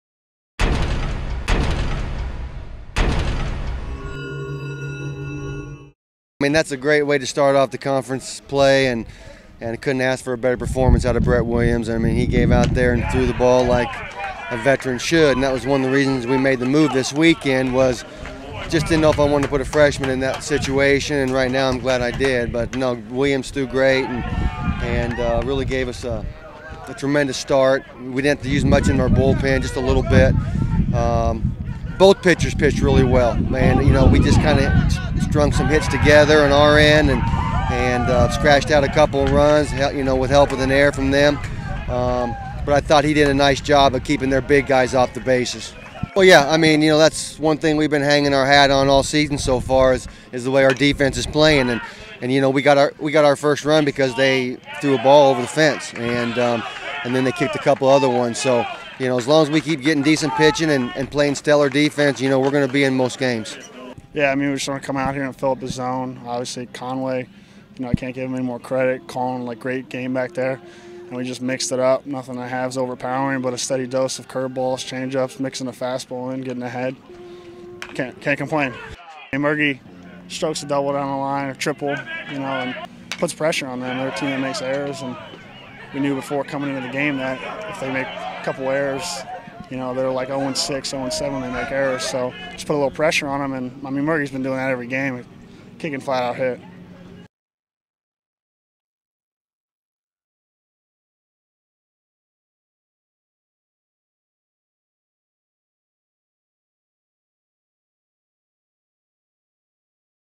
SpiderTV: Baseball Blanks Bonnies In A-10 Opener
The Richmond baseball team shutout St. Bonaventure, 3-0, Friday at Pitt Field behind sophomore lefty Bret Williams gem on the mound,